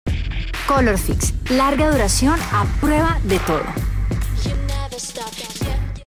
Colorfix: Labial Duo Tattoo a prueba de todo
Greeicy pone a prueba Colorfix, nuestra línea de maquillaje de larga duración a  prueba de TODO que se volverá tu favorita.
¿Ya la probaste?